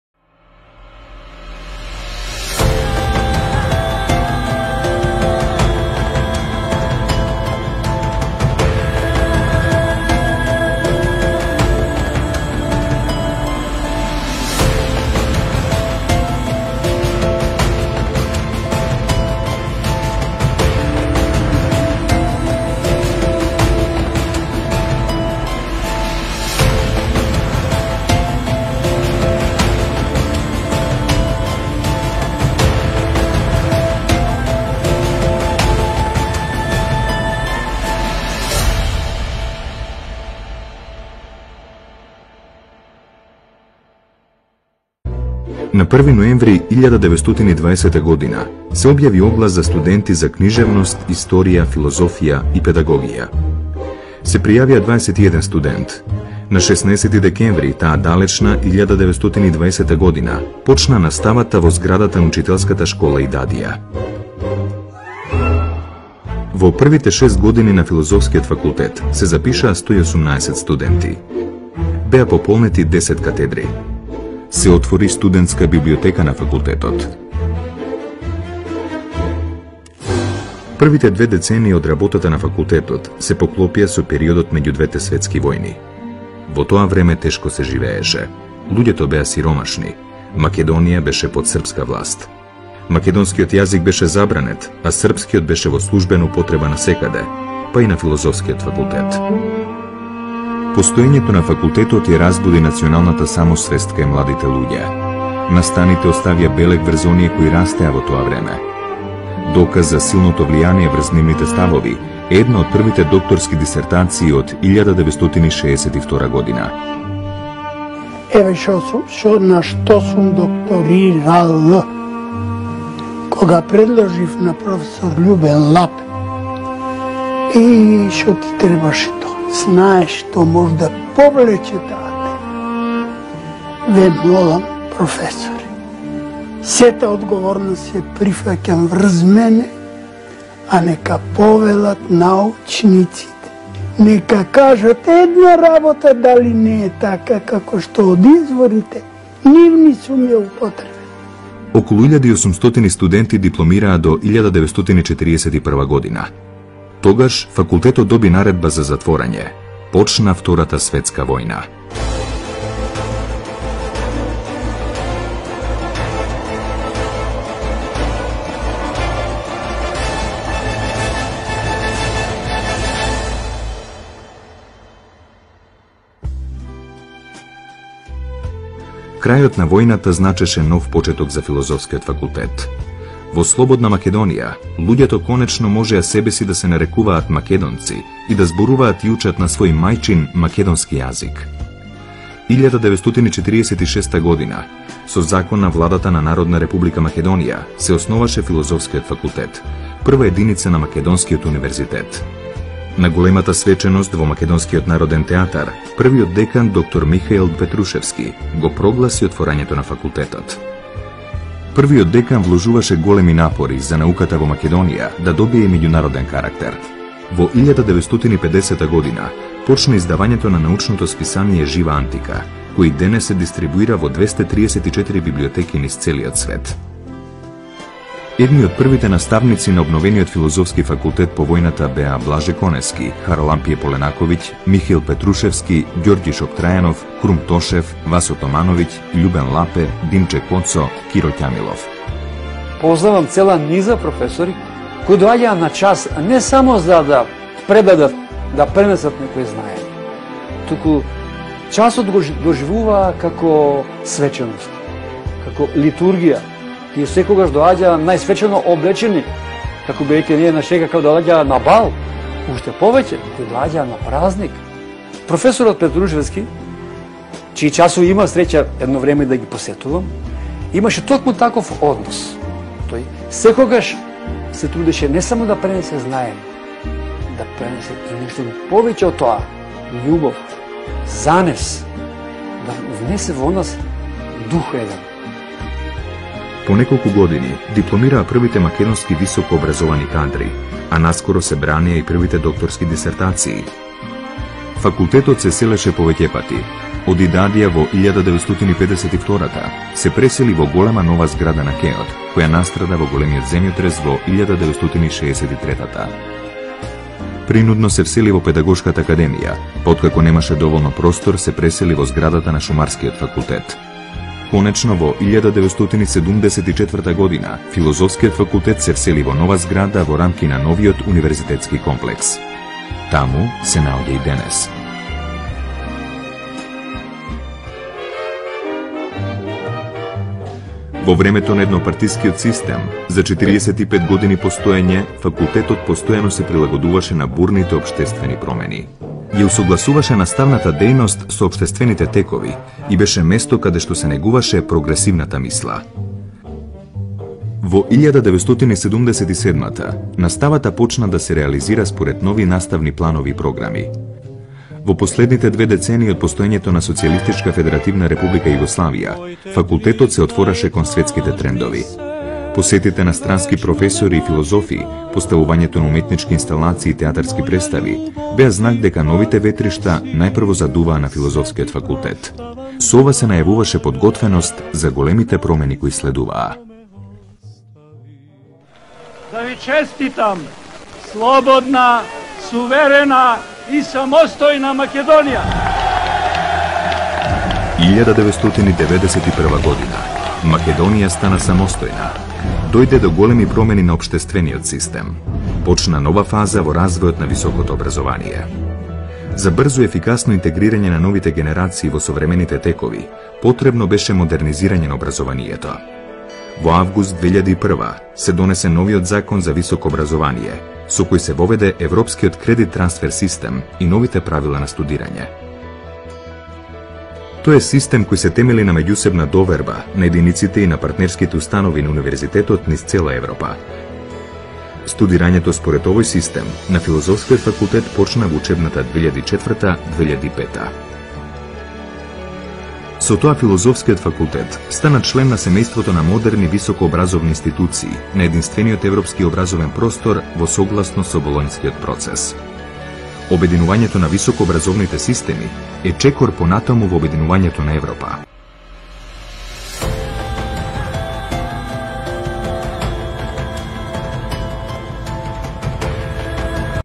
Faculty of Philosophy short intro
Short documentary about the foundation and development of the Faculty of Philosophy in Skopje.